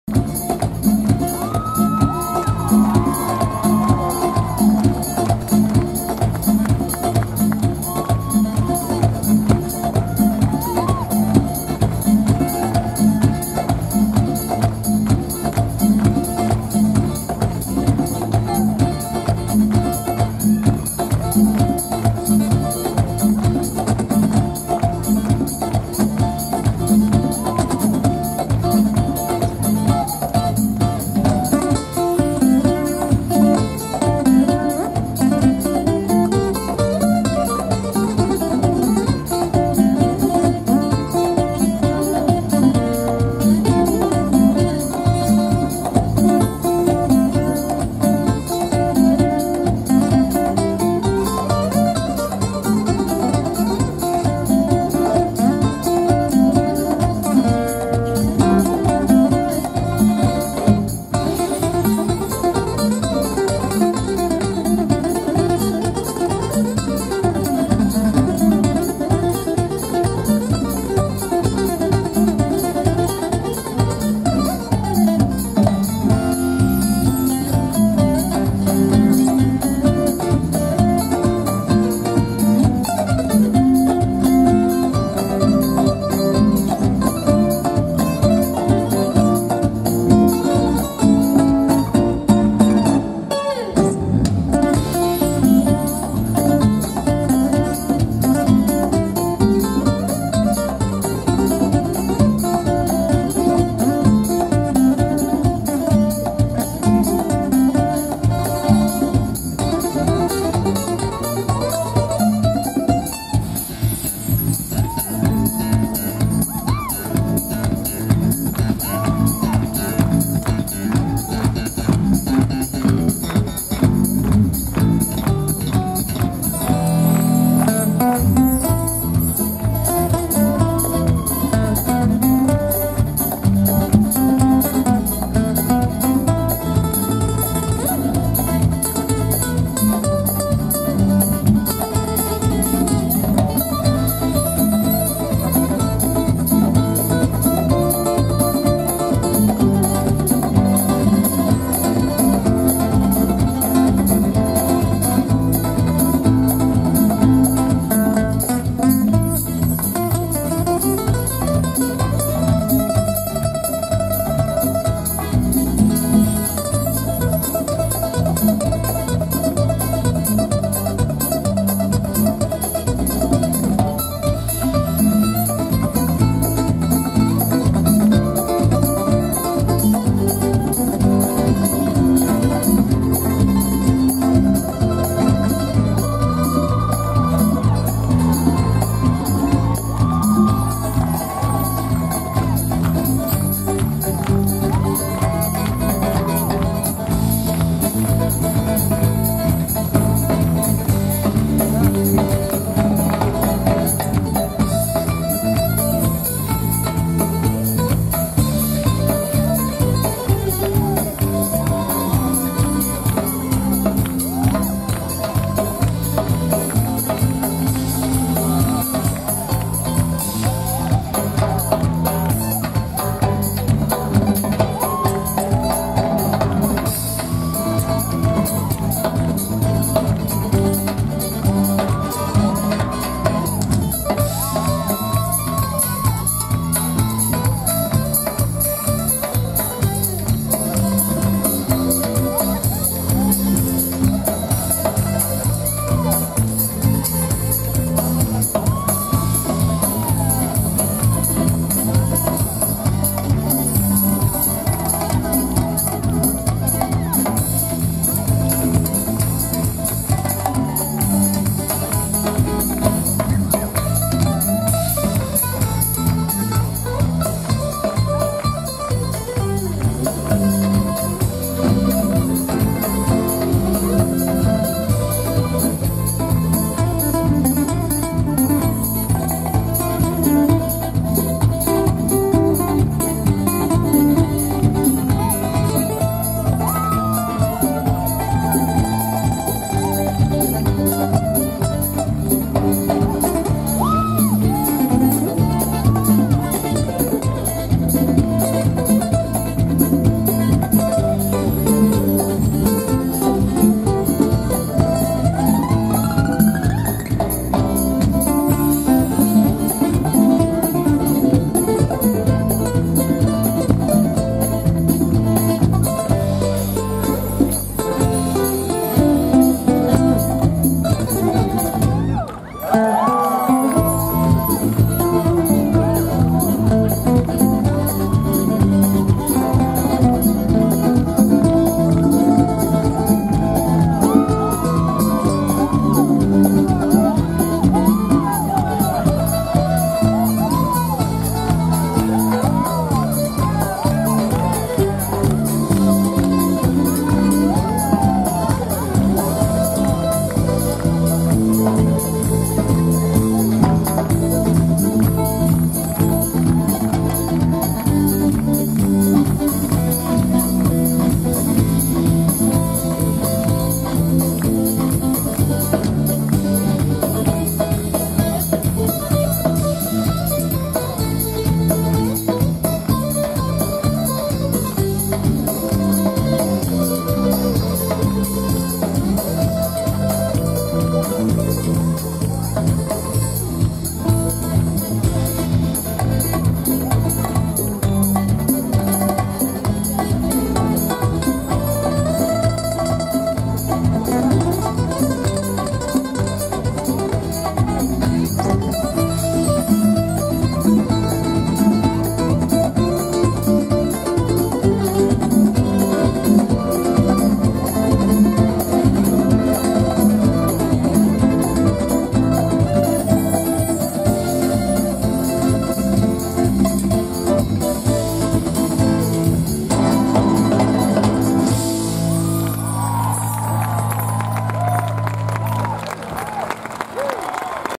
Pavlo at CNE 3017
Pavlo play at the Toronto CNE Bandshell on Monday August 28, 2017